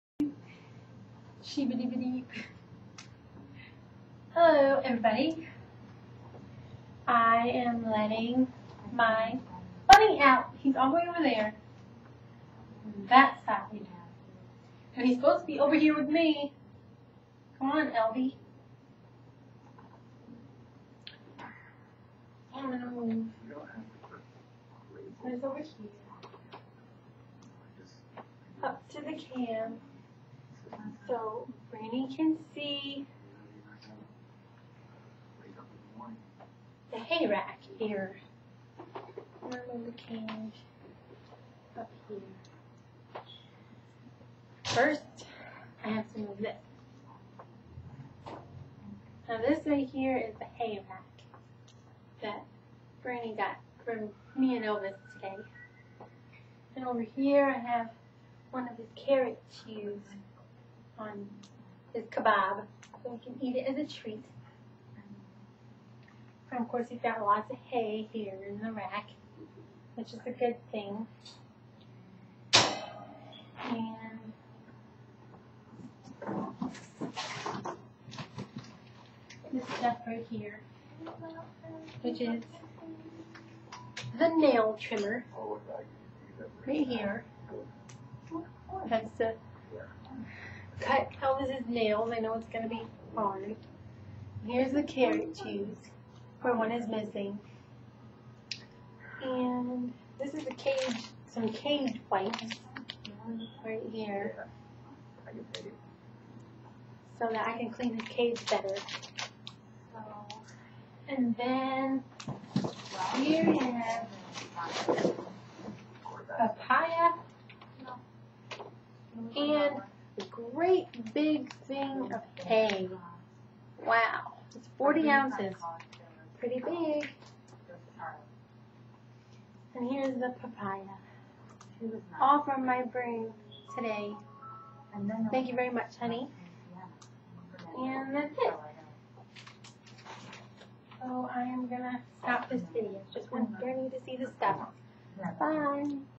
What Bern Got 4 Elvis!! ♥
This is all the stuff my boyfriend Bernie brought for my Bunny, Elvis!! ♥